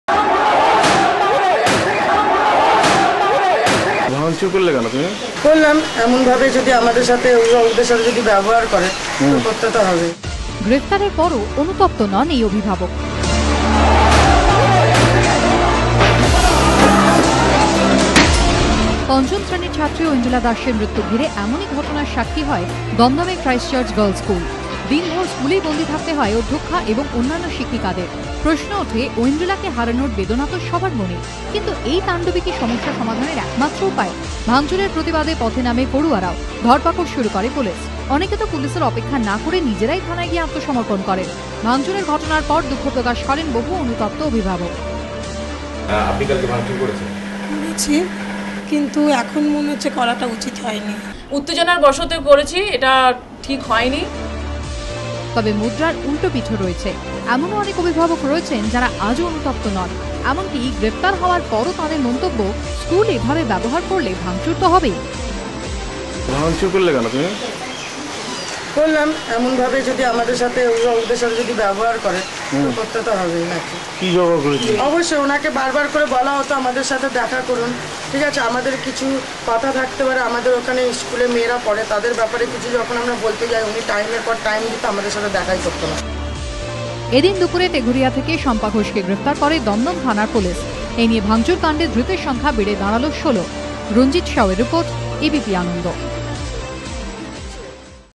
Arrests continue in the incident of ransacking of christ church school in dumdum.
Arrests continue in the incident of ransacking of christ church school in dumdum. Some guardians after arrest expressed regret for participating in hooliganism, but some still show no remorse for it.